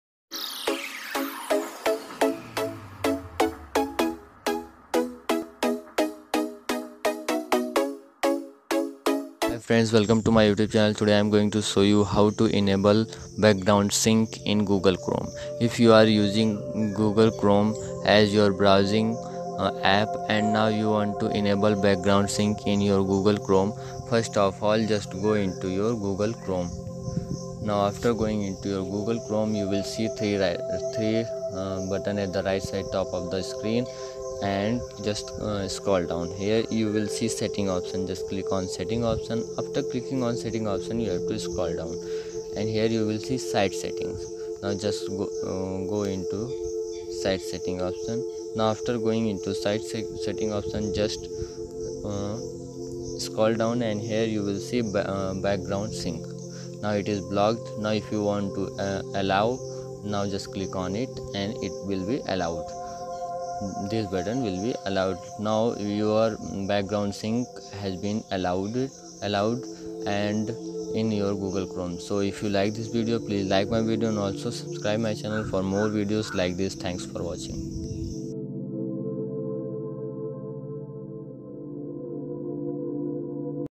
How To Enable Background Sync In Google Chrome
Hi Friends, In this video, I am going to show you how to Enable Background sync in google Chrome. If you are using google Chrome and you haven't enable background sync in Google Chrome, you can easily enable it from your Google Chrome Setting. After enabling google Chrome background sync, Google chrome will sync all your data and whenever you need it, you can recover it from google chrome.
1. First of all, Go to Google Chrome app.
2. Now click on Three dot at the right side corner of the screen.
3. Now scroll down and click on the setting option.
4. Now again you have to scroll down.
5. Here you will find site setting option.
6. Click on the sete setting option and open it.
7. Scroll download and you will see back ground sync option.
8. Now click on it and allow background sync.
So like this you can enable background sync in google Chrome app.